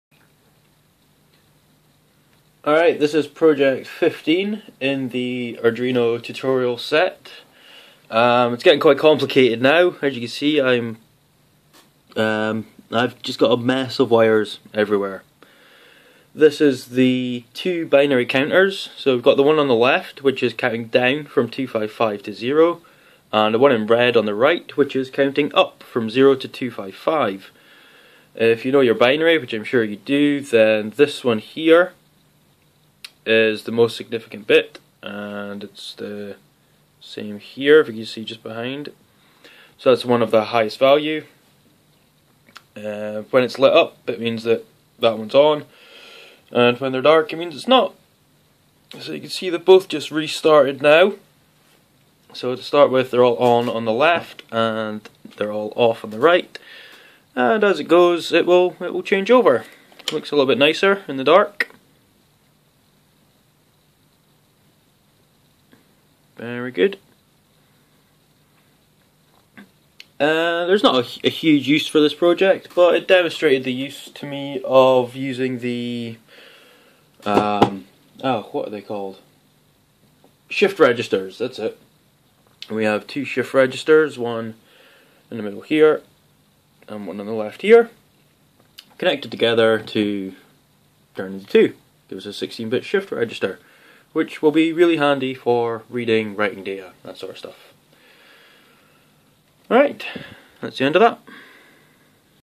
Arduino - 16bit Shift Register Powering 2 Binary Counters
A short video of one of the example projects I'm working through to get to grips with the Arduino.

This is Project 15 of the tutorial projects provided by Earthshine Electronics (website has since went down).